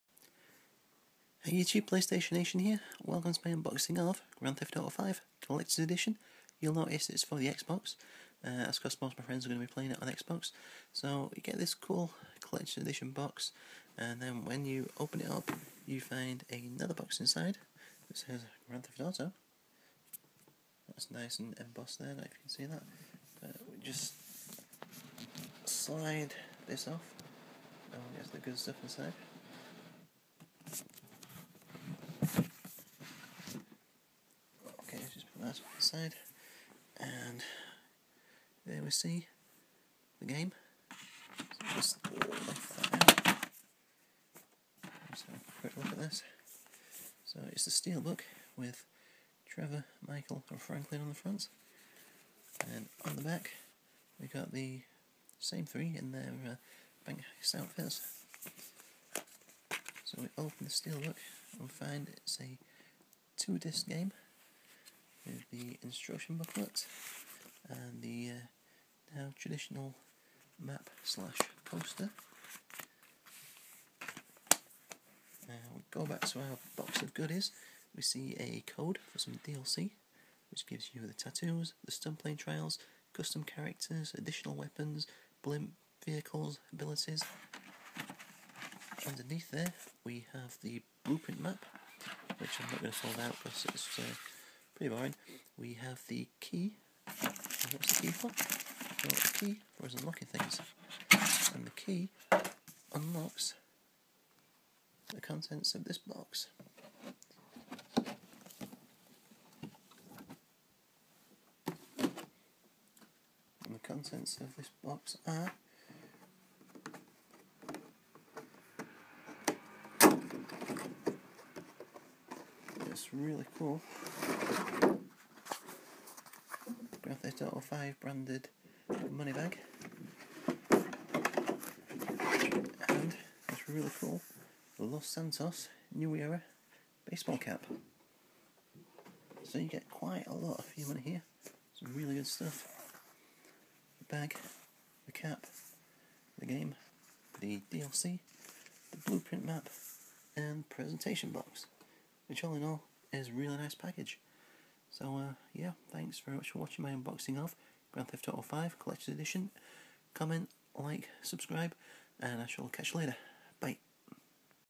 GTA 5 Collectors Edition Unboxing Xbox 360
My unboxing of the collectors edition of grand theft auto 5 for the Xbox 360